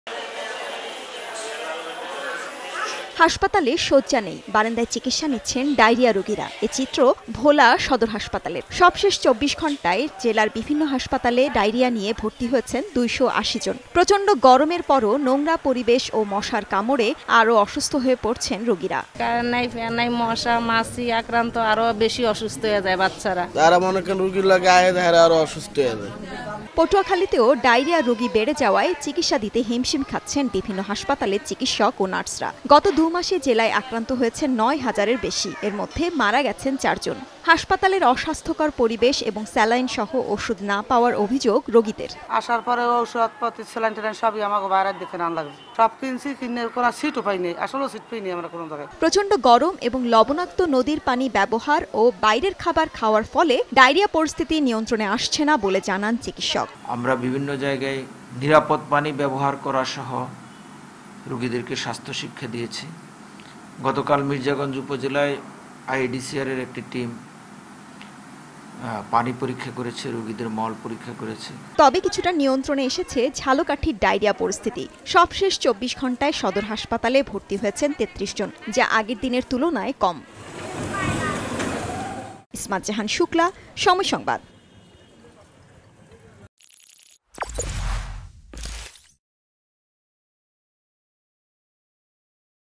কোনভাবেই নিয়ন্ত্রণে আসছে না ভোলা ও পটুয়াখালীর ডায়রিয়া পরিস্থিতি! | Diarrhea News | Somoy TV
ঝালকাঠিতে কিছুট উন্নতি হলেও, ভোলা ও পটুয়াখালীর ডায়রিয়া পরিস্থিতি কোনভাবেই নিয়ন্ত্রণে আসছে না। প্রতিদিন ধারণ ক্ষমতার চেয়ে কয়েকগুণ বেশি রোগী ভর্তি হচ্ছেন সরকারি-বেরসারিক বিভিন্ন হাসপাতালে। দেখা দিয়েছে শয্যা ও ওষুধ সংকট। প্রচণ্ড গরম এবং অসাবধানতার কারণেই ডায়রিয়ার প্রাদুর্ভাব বলে জানান চিকিৎসকরা।